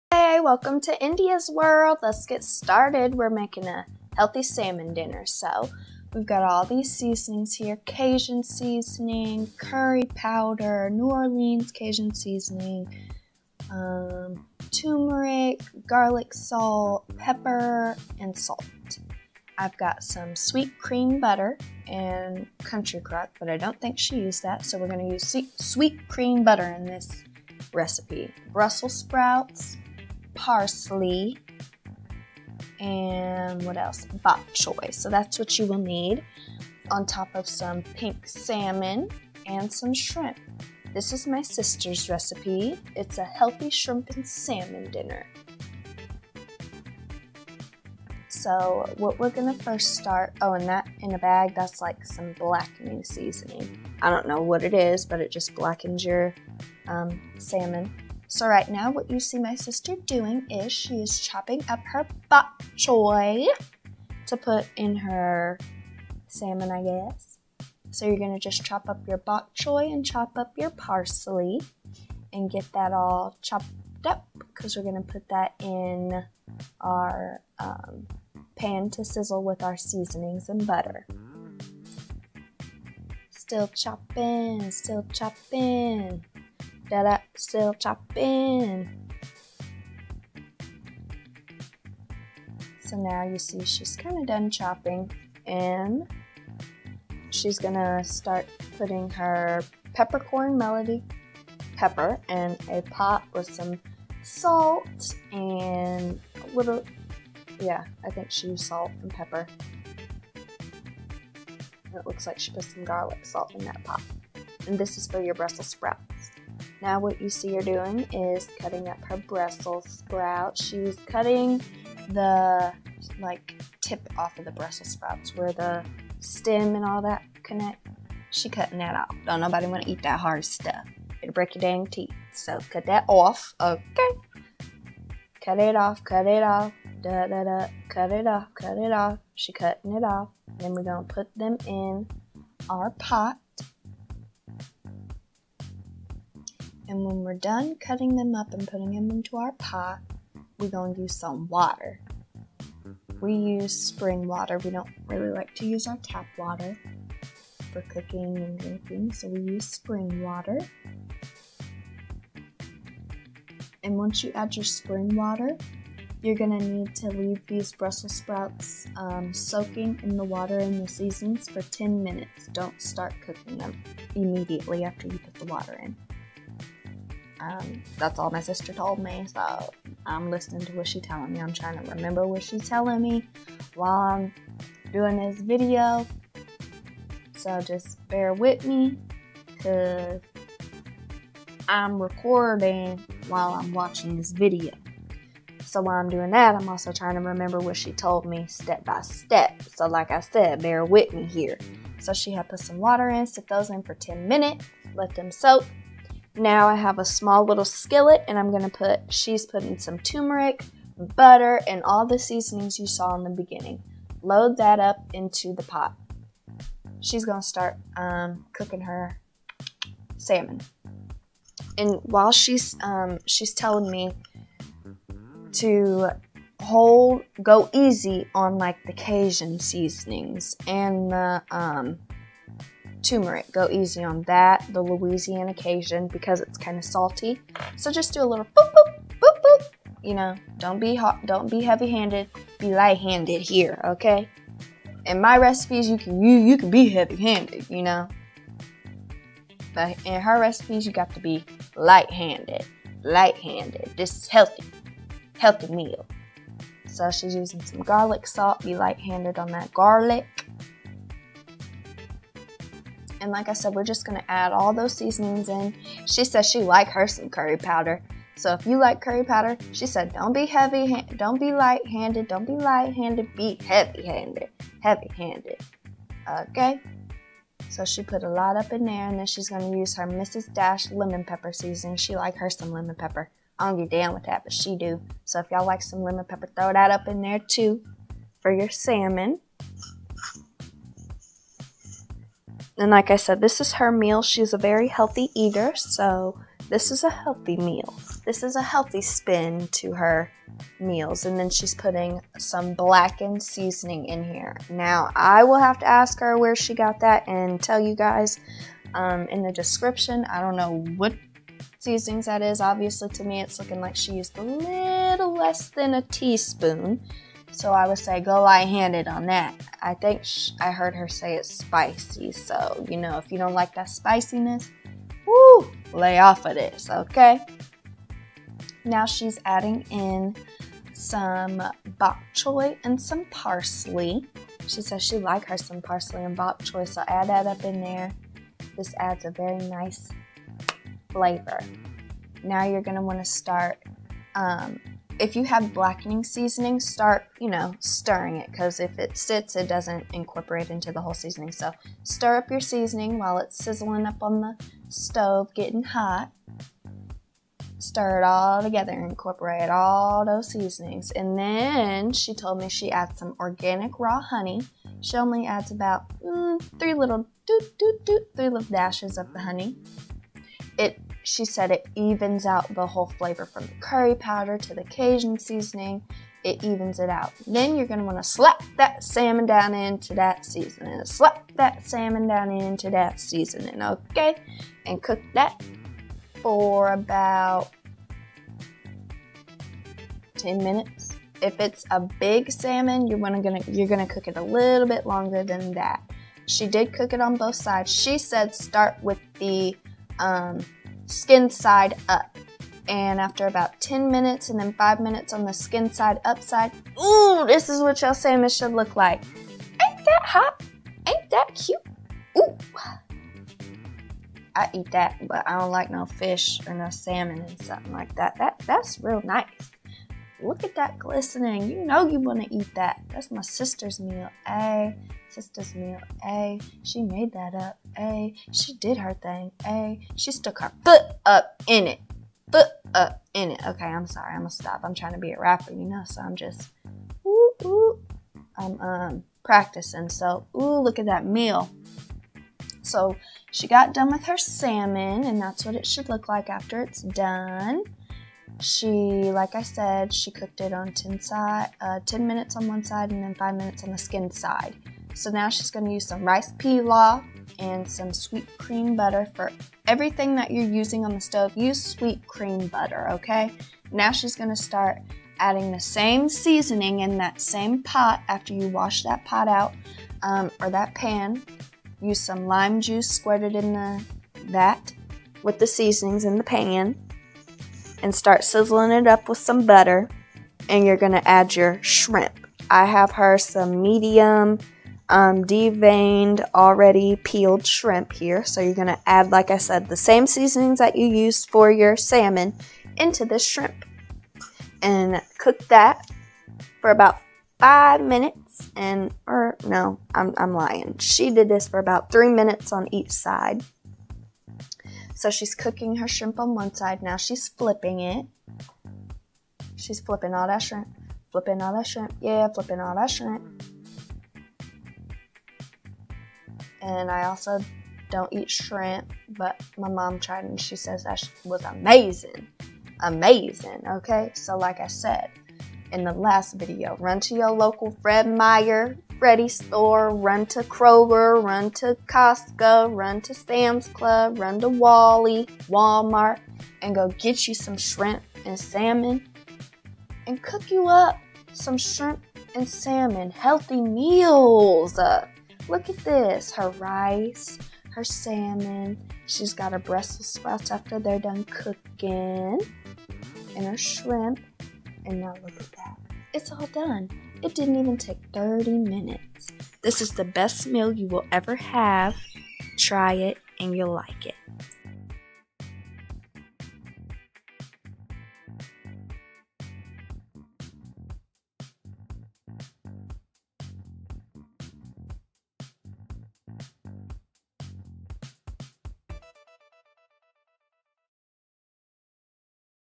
Healthy Salmon & Shrimp Dinner
Ingredients:
1 Pink Salmon
8 Deveined Shrimp
7 Fresh Brussel Sprouts
1 Pack of Uncle bens Rice Pilaf
1 Bag of Cilantro to your liking
1 Bag of Bok Choy add much as you like
2 Organic Limes
4 drops of Organic Raw Honey for the Salmon
Seasonings:
Blackening seasoning from your local grocery store
Cajun spices and herbs:
N'Orelans Cajun seasoning
Lousiana Cajun seasoning
Dash Lemon Pepper
Curry Powder
Peppercorn Medley Pepper
Salt
Ground Turmeric
Garlic salt
(All the seasonings go on both, the salmon and the shrimp)
For best taste wash your Brussel sprouts, cilantro, and bok choy with Fit organic fruit and vegetable spray. At your local Walmart .
Wash your Brussels sprouts and add them to cold saltwater for 10 mins in a bowl and set aside. After ten minutes add the Brussel sprouts and 1 tbsp of butter to a pot of water and boil on Medium-High until they are soft. In the meantime check them with a fork to check the softness. When the Brussel sprouts are done you can add your favorite seasonings to them.
Prep the salmon and the shrimp with the seasonings listed above
and Cut up your Bok Choy and cilantro to add to the salmon and shrimp before and after they are done.
After you have prepped the salmon and shrimp you can add 2 tbsp of butter, bok choy, lime juice, cilantro, and garlic salt to the skillet for about 2 mins. Then add the Salmon to the same skillet and cook on both sides for 5 mins (Dependent on how big your salmon is). Once the salmon is cooked add 4 to 5 drops of organic raw honey to the skillet (Just for the salmon).
For the shrimp: Add 2 tbsp of butter, bok choy, lime juice, cilantro, and garlic salt to the skillet for about 2 mins. Then add the shrimp to the skillet and cook the shrimp on both sides for 4 mins.
Place the packet of Uncle Ben's rice in the microwave for 1 min or until soft and add cilantro into the bag as well.
After all the food is done plate it and cut up some limes and place them on the plate and use more limes to drip lime juice on the shrimp, rice, Brussel sprouts, and salmon.